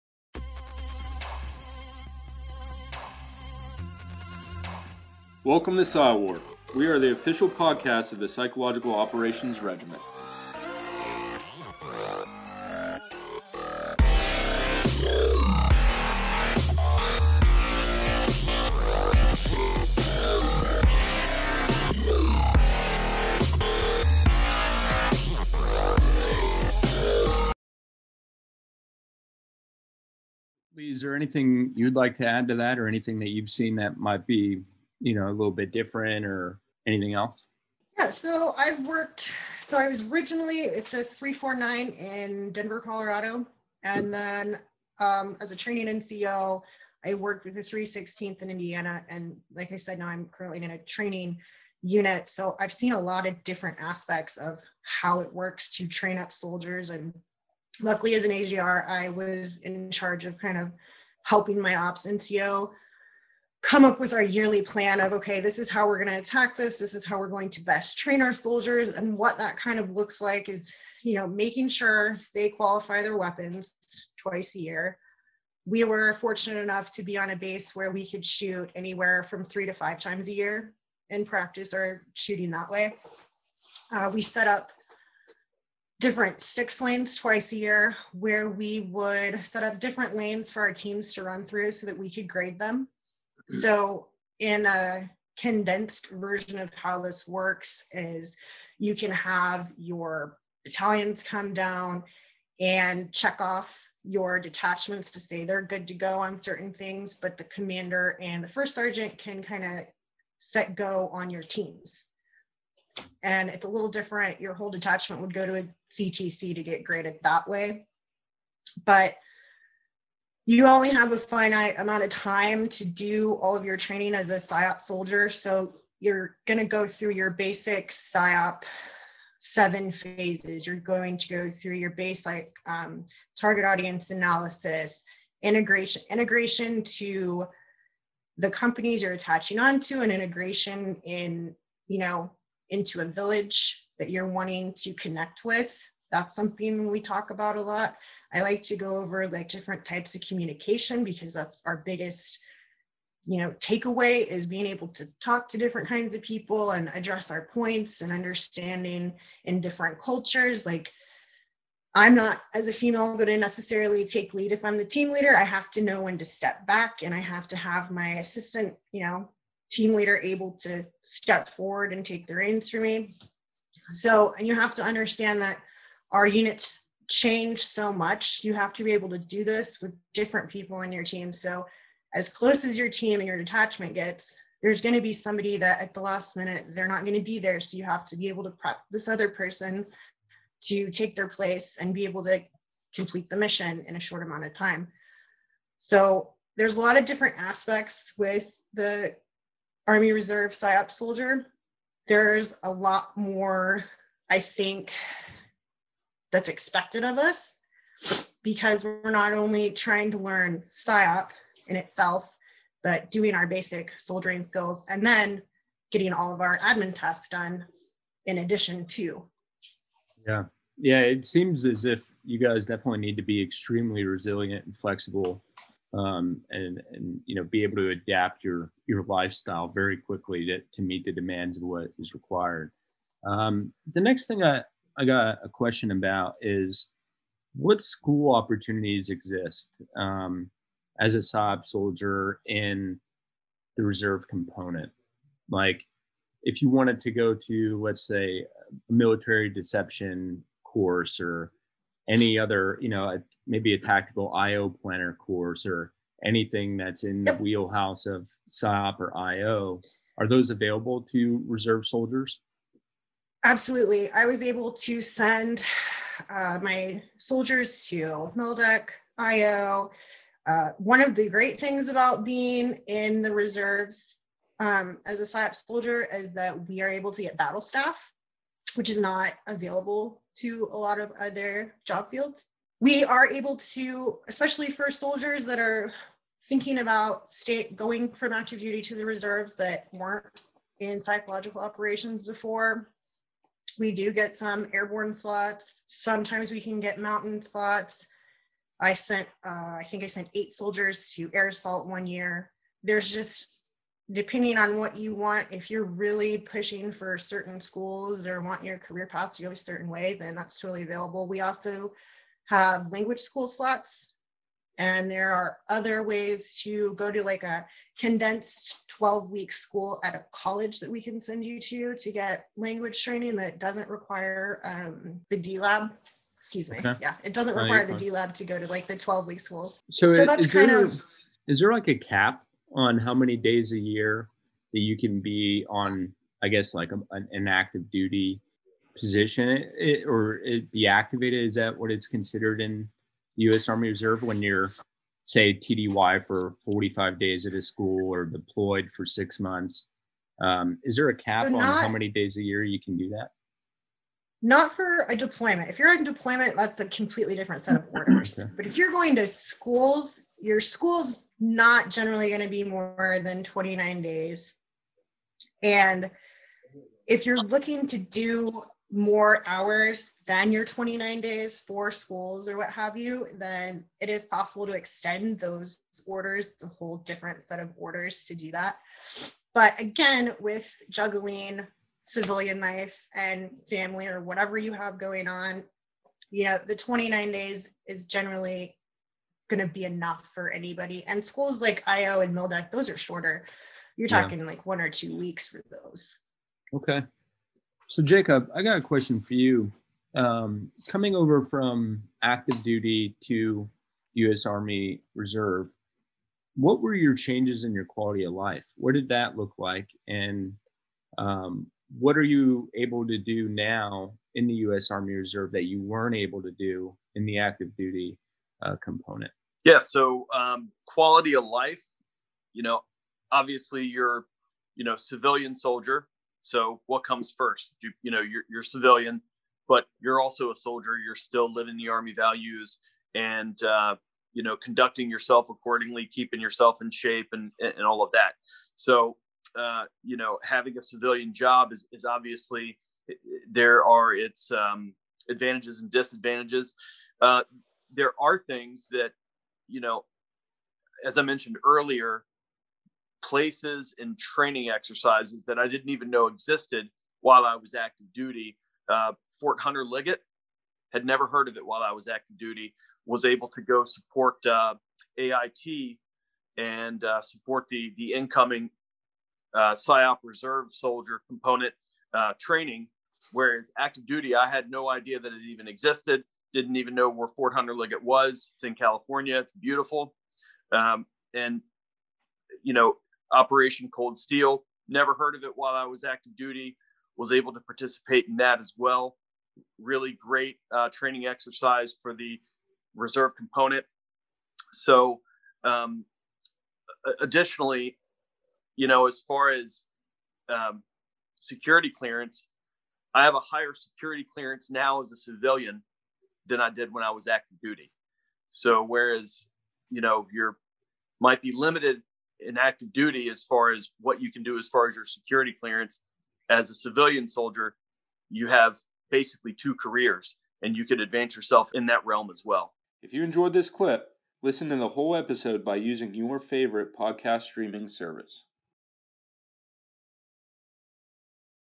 The World of PSYOP Reserve
The 8th Psychological Operations Group (Airborne) speak with Staff Sergeant Jacob Strickland and Staff Sergeant Leigh Nauman from the U.S. Army Reserve.  In this episode both U.S. Army Reserve Staff Sergeants dive deep into the both the options and benefits of enlisting into the U.S. Army Reserve PSYOP.  To find out more, listen in to "The World of PSYOP Reserve”!

Or speak with your Reserve Officer Career Counselor at your Soldier Support Center or transition point.